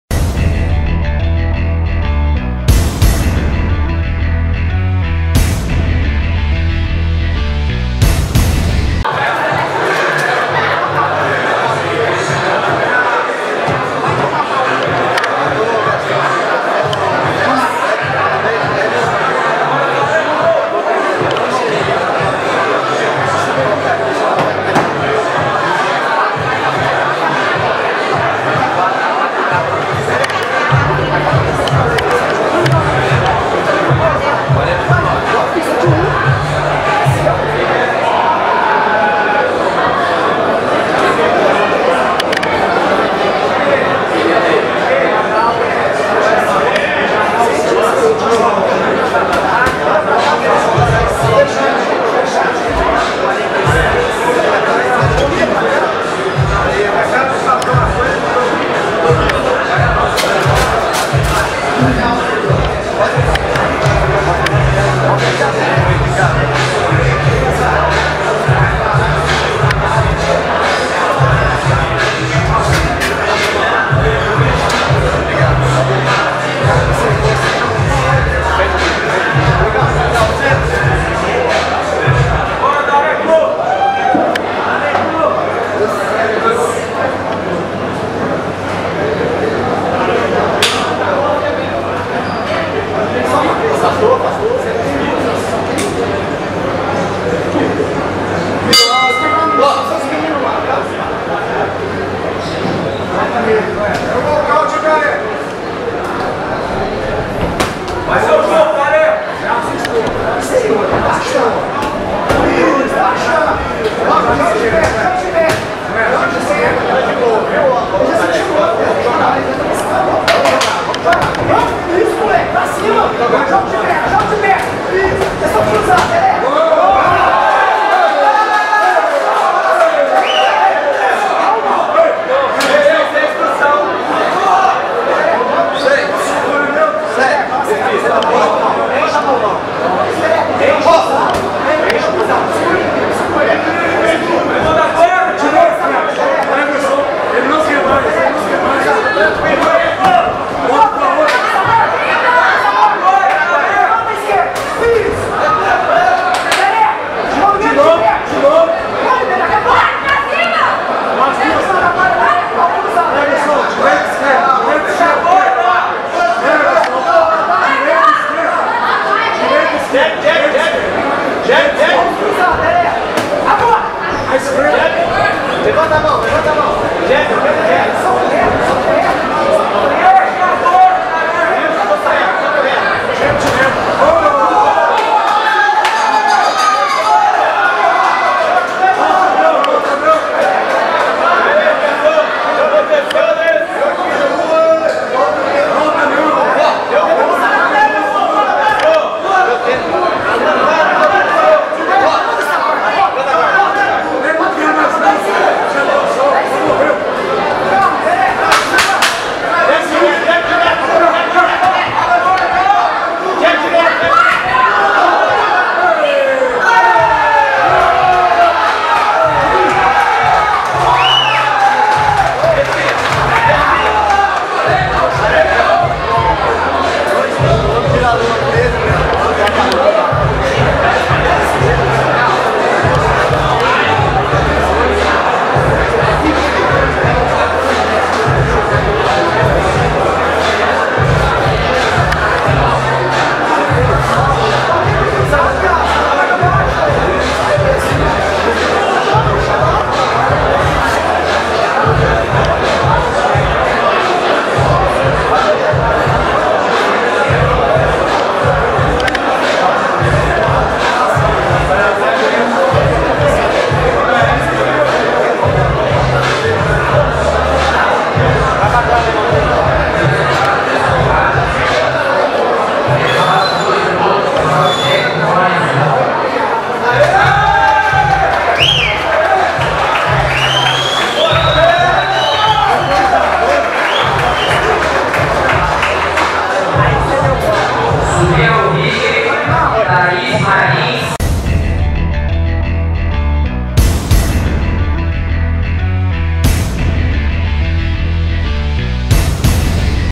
Gustavo vs Cleverson - PBoxe 5
Em combate de Boxe até 75kg, Cleverson Cabelo (Akio Matsuura) venceu Gustavo Tareco (Cia Fight) por nocaute técnico.